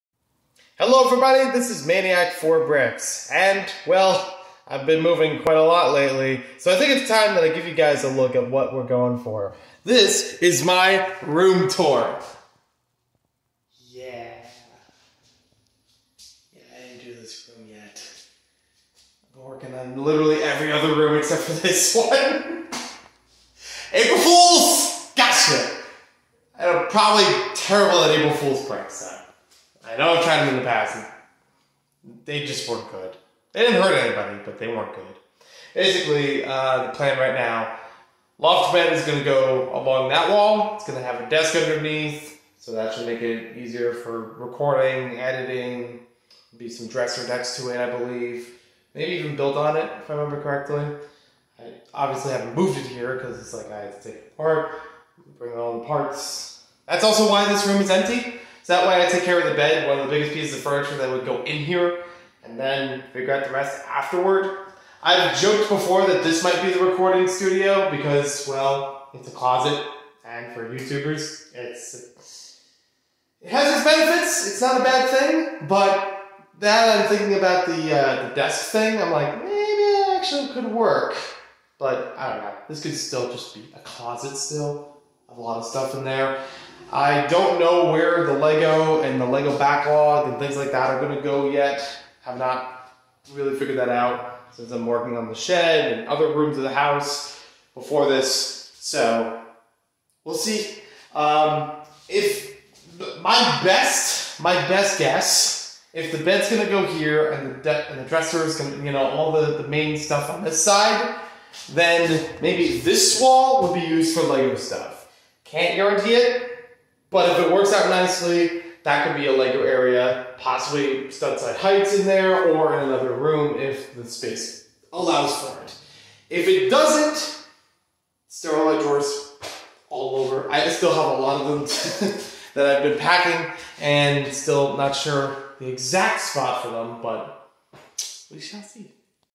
New House ROOM TOUR!!
I recorded this over a week ago as one of several ideas for an April Fool's prank. I don't care it's uploaded a day late. I explain in the video why it's empty, why don't you watch and find out? 2-minute retention time jeez YouTube.